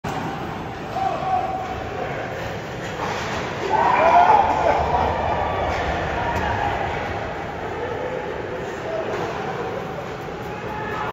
Ryan Leonard goal at Capitals Development Camp
Ryan Leonard scores in the Capitals 2024 Development Camp scrimmage. Leonard's Team White won the 3-on-3 tournament.